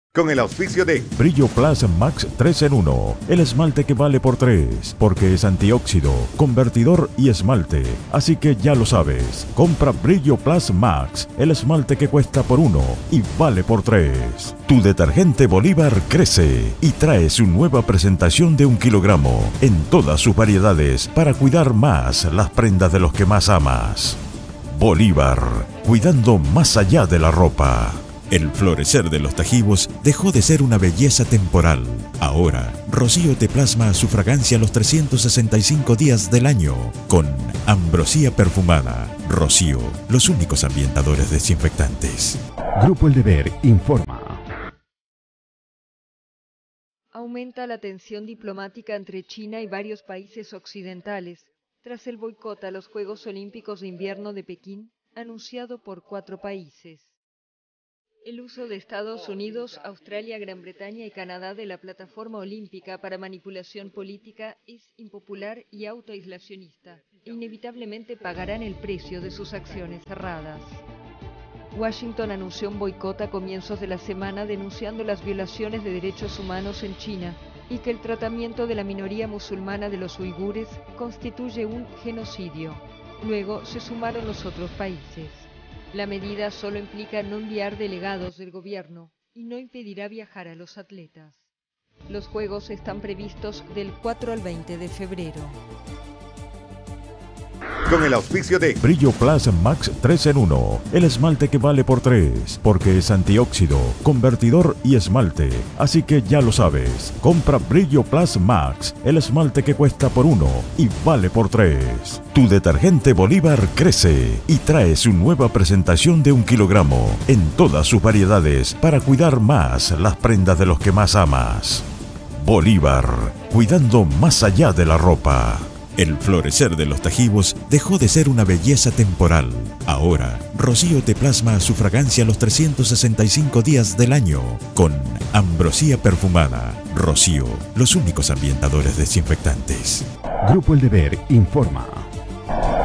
BOICOT DIPLOMÁTICO DE JUEGOS DE PEKÍN
China advierte a países que impulsan boicot diplomático de Juegos de Pekín que "pagarán" el precio